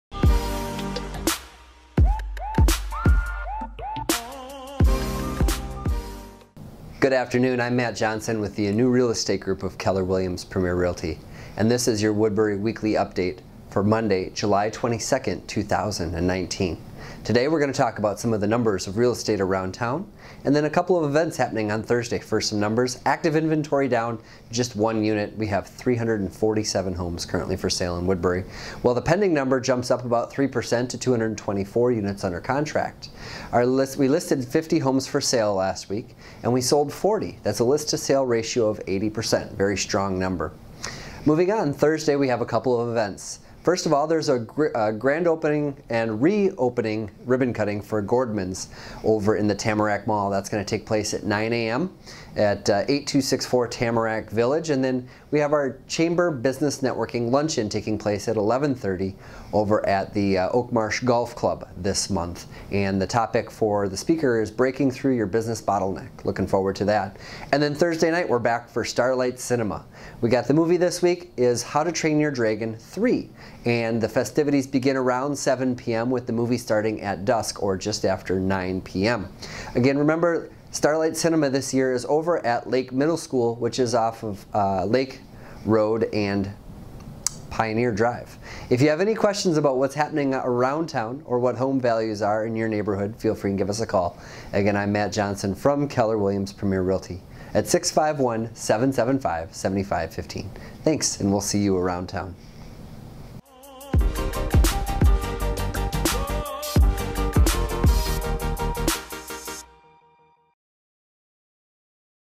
Woodbury Weekly Update for July 22nd, 2019
In this Weekly Update we give you some upcoming events on Thursday from Gordmans, Chamber of Commerce, and Starlight Cinema. We also like to give you info on the real estate market to help you stay updated.